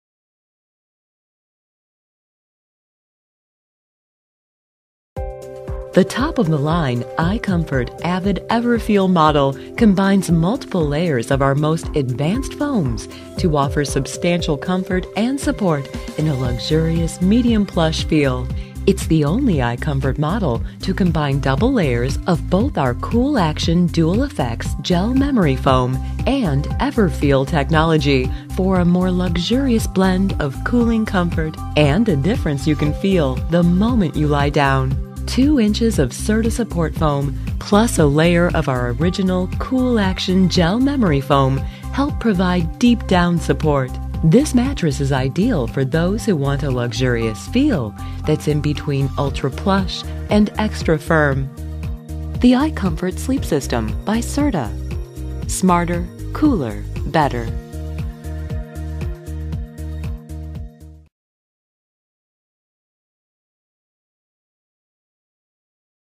Serta Mattress - Icomfort Avid Everfeel by GreatFurnitureDeal com
Since its introduction, the iComfort® Sleep System has helped people get the comfort they need without the potential negatives associated with traditional memory foam sleep systems. In fact:

    98% of iComfort mattress owners surveyed are satisfied with their mattress purchase
    97% would recommend iComfort to a friend or colleague
    9 out of 10 iComfort mattress owners agree that iComfort properly supports their entire body

(Source: GFK Buyer Satisfaction Study, 2012)

Now, we are introducing all-new iComfort models for 2014 with even more innovations designed to provide you with the cooling comfort and support you need for a rejuvenating sleep experience. Every new iComfort mattress now features:

    Cool Action™ Dual Effects® Gel Memory Foam – Serta’s latest breakthrough in gel memory foam technology.
    EverFeel™ Technology for a difference you’ll feel the moment you lie down

We offer a variety of models ranging in feel from luxuriously plush to extra-firm so you can find your comfort match! Shop our iComfort mattress models now.